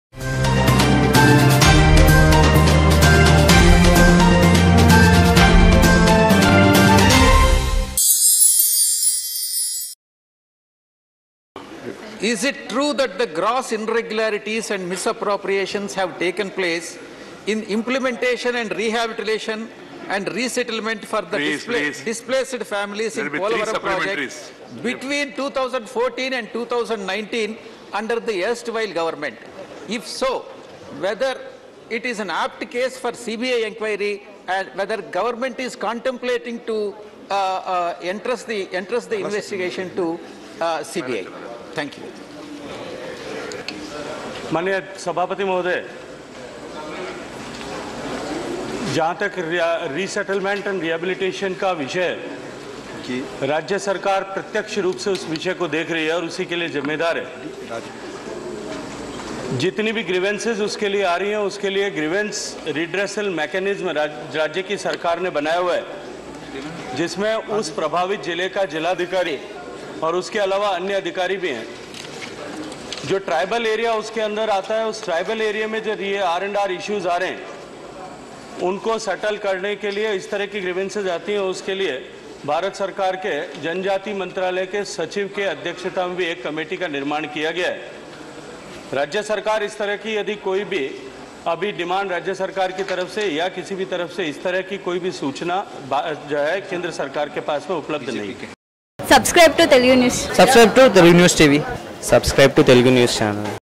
YSRCP MP VijayaSaiReddy On Polavaram | RajyaSabha || Telugunewstv
Like and follow @TeluguNewsTV, News channel with exclusive Breaking news, Special Interviews, latest updates on Politics, Movies, Sports and Current Affair